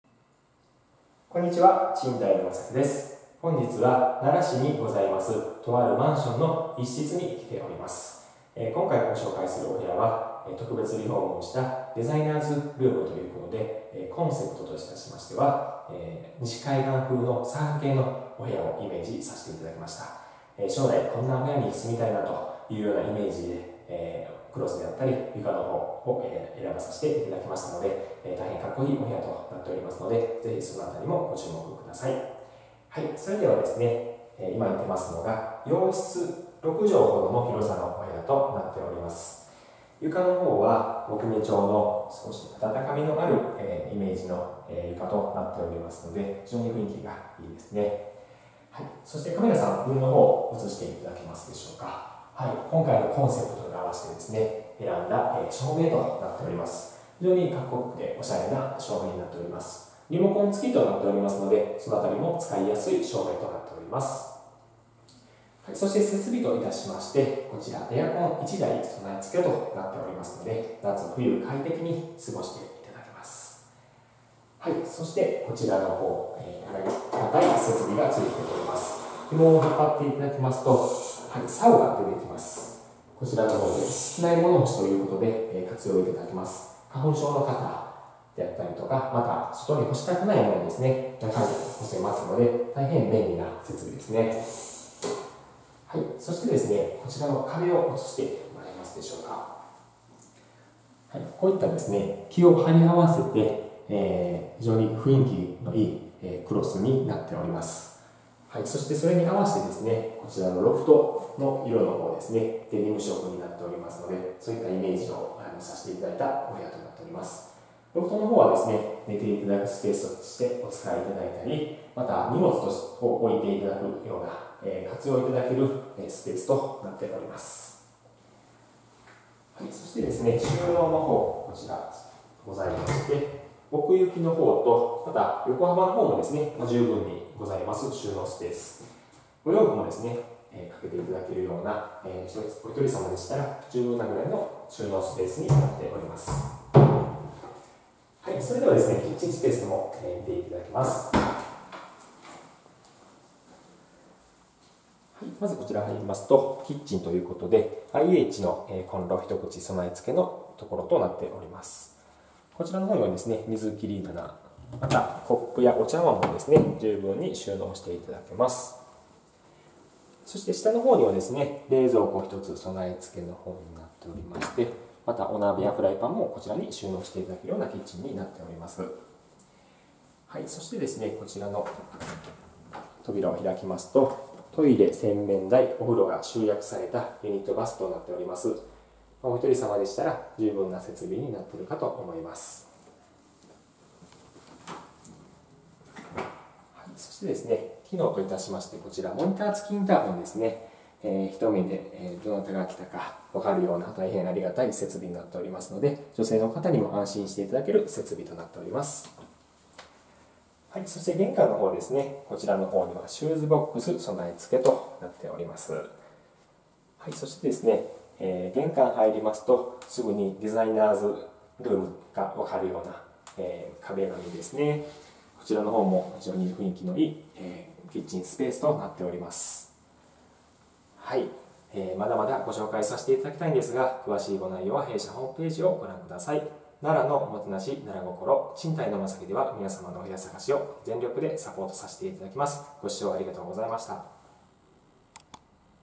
【ルームツアー】マンション福徳｜奈良市尼ヶ辻駅賃貸｜賃貸のマサキ｜Japanese Room Tour｜001264-1-7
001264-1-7
奈良県奈良市三条大路３丁目1-9にあります評判の良い間取りの1Kマンション【マンション福徳】の物件紹介動画です。

【最寄り駅】
近鉄難波・奈良線「大和西大寺」駅 徒歩29分